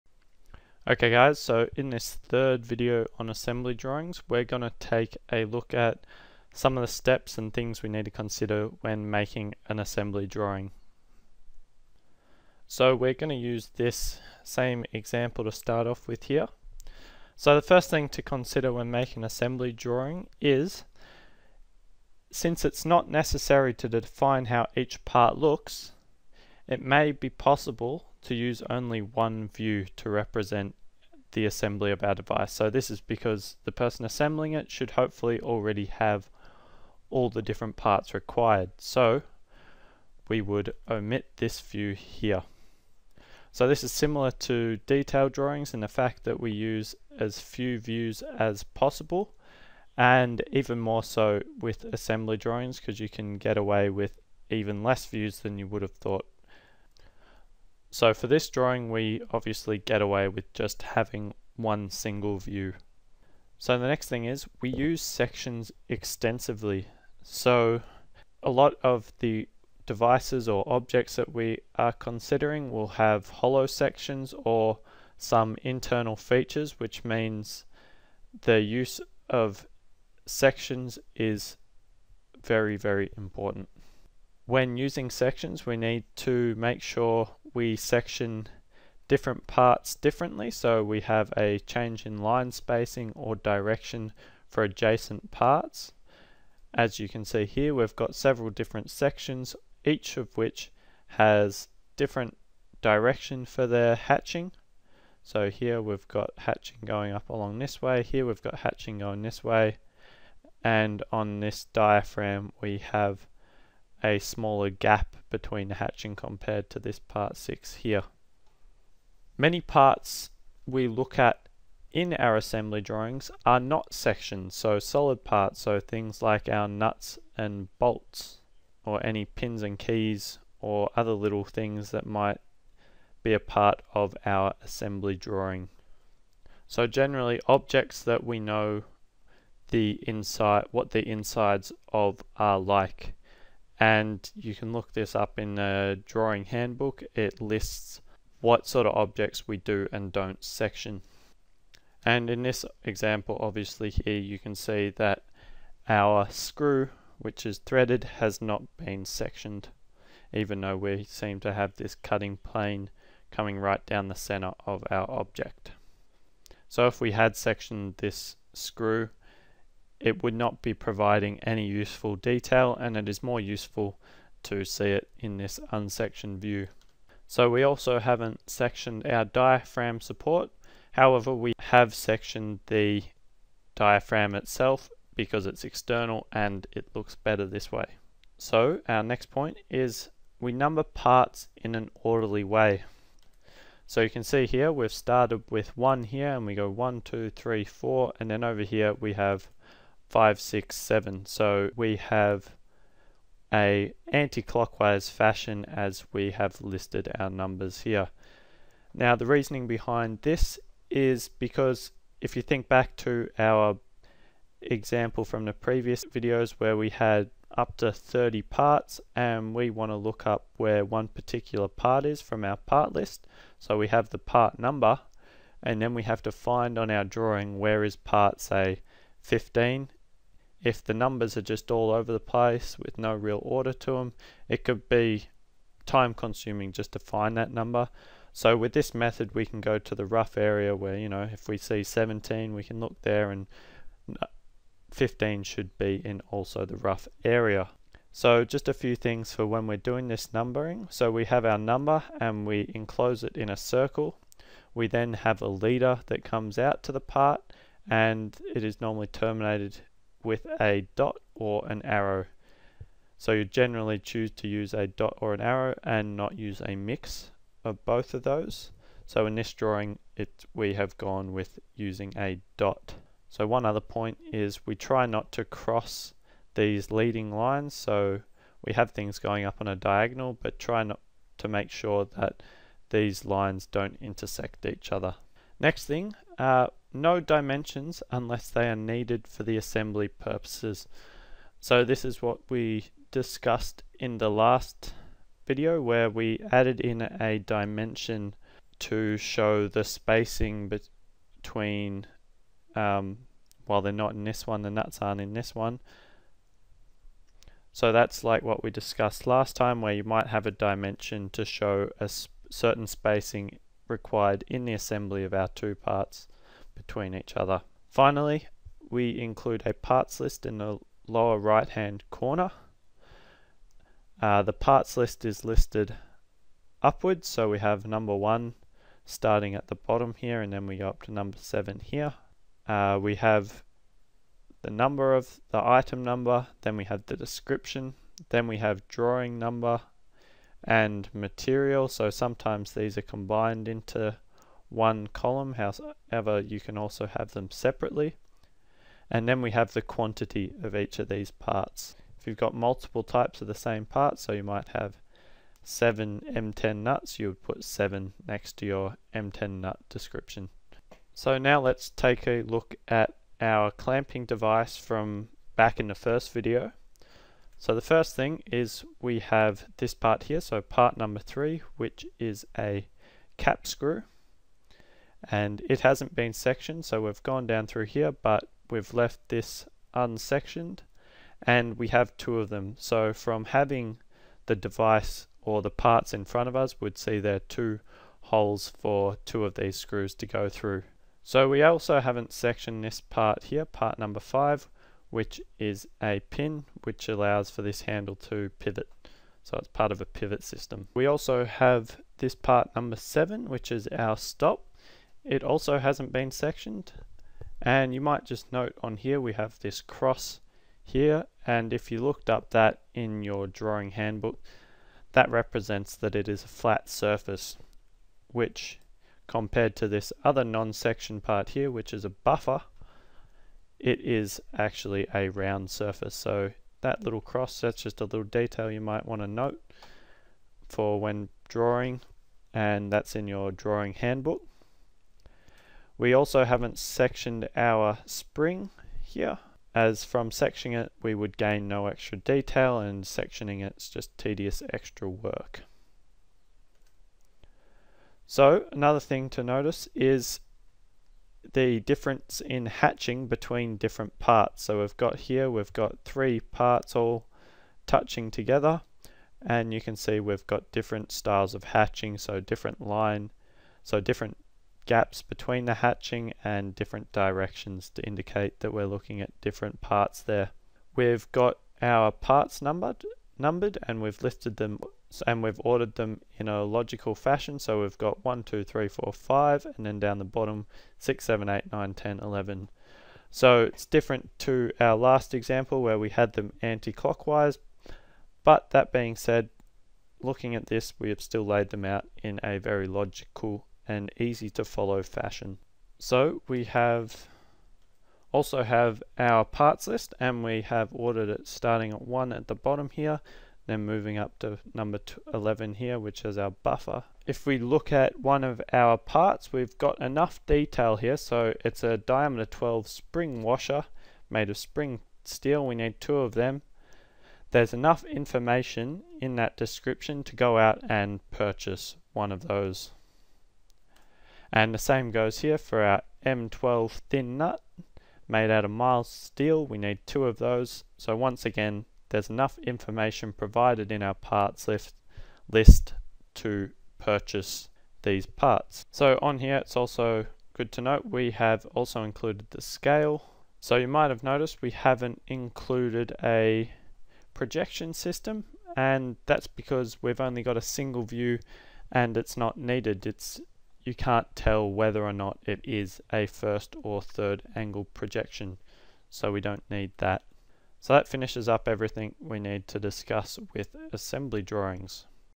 Assembly Drawings 3
This video explains how to go about producing an assembly drawing